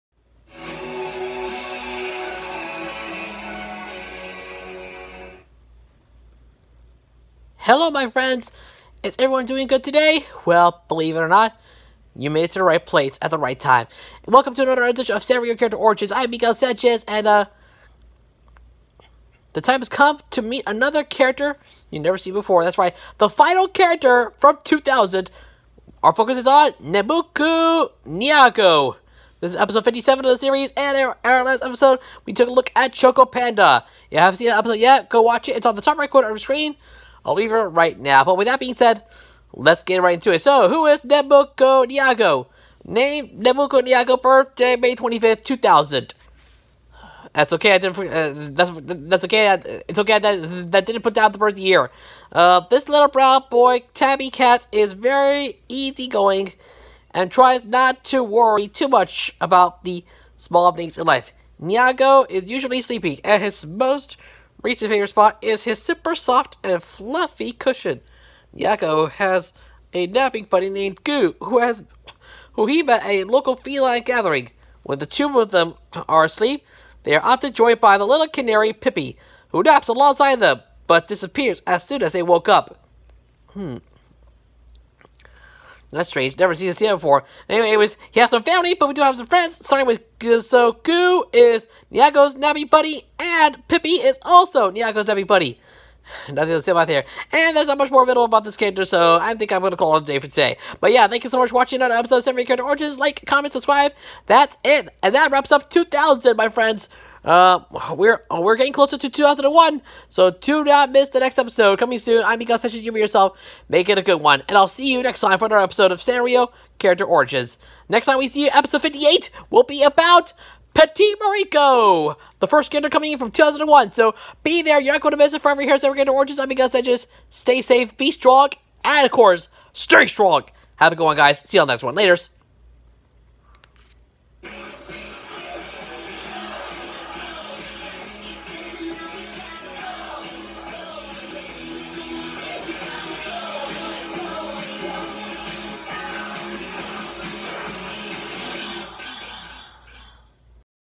Sanrio Character Origins #57 - Nemukko Nyago
Hello, YouTube! Welcome to Episode 57 of Sanrio Character Origins! In this episode, We take a look at the origins of Nemukko Nyago! Enjoy, guys!